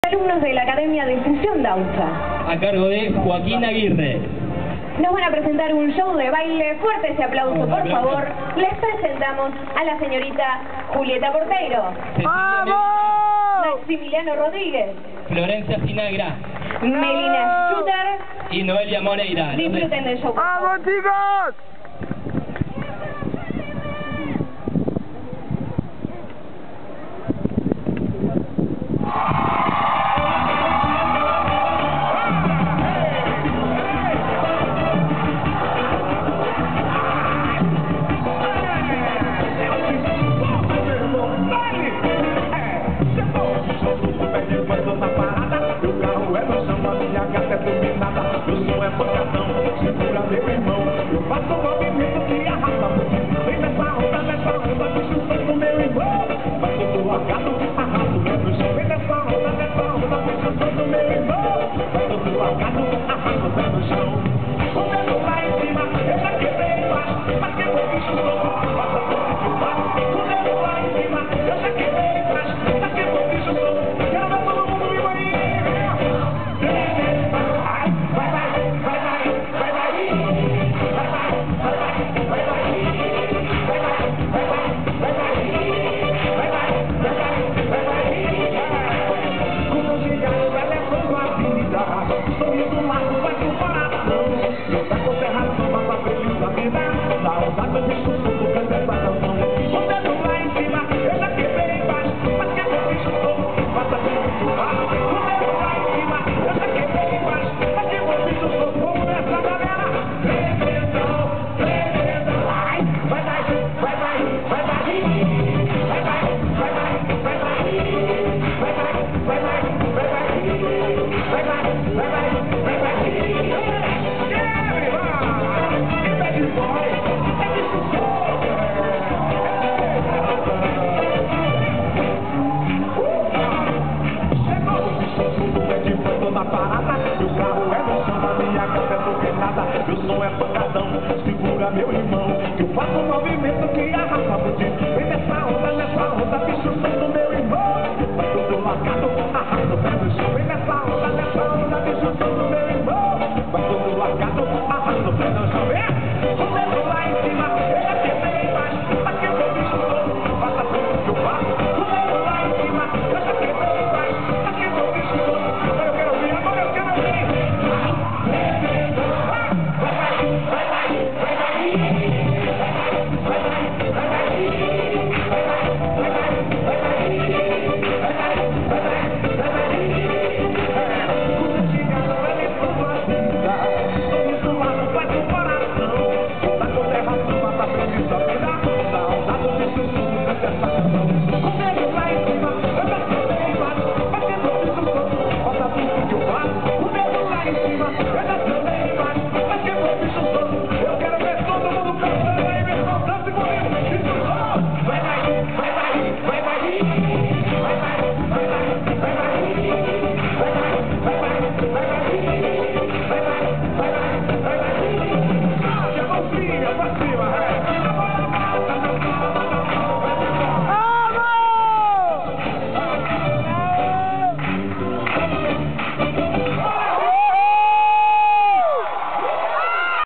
Cia Movimiento y ritmo - Show de Axé en el Spring Day Quilmes
junto a mis alumnos de la Academia Fusion Danza fuimos invitados a participar en el Spring Day Quilmes, una feria de diseño realizada en el Club Quilmes. Gracias a los organizadores por todo y a mis alumnos una vez mas...